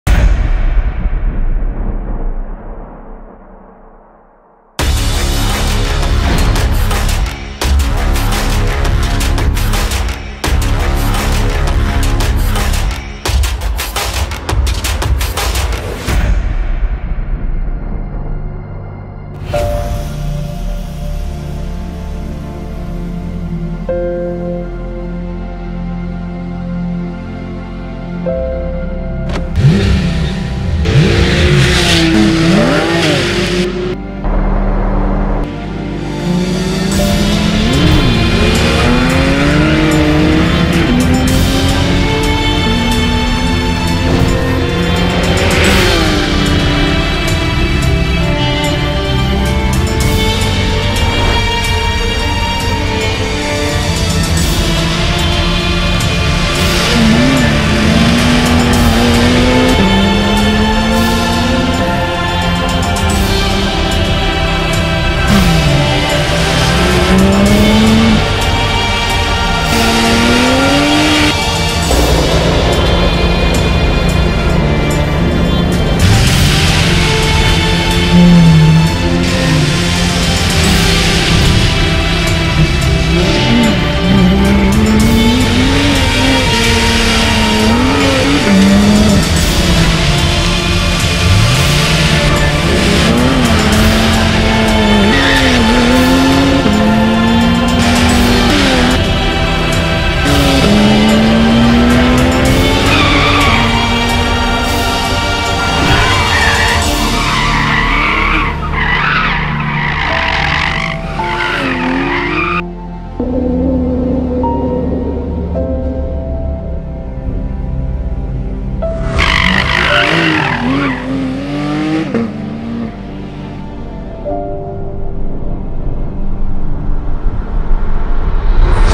Silesia Ring Porsche 911 Cup Car Promotional Video
Car promotional video. Porsche 911 Cup test drive at Silesia Ring.

Silesia Ring, Poland, March 2019
Film by: Adam Fabian Szewczyk / Superstudios.pl
Driver: Marcin Fedyna/PMshooter.pl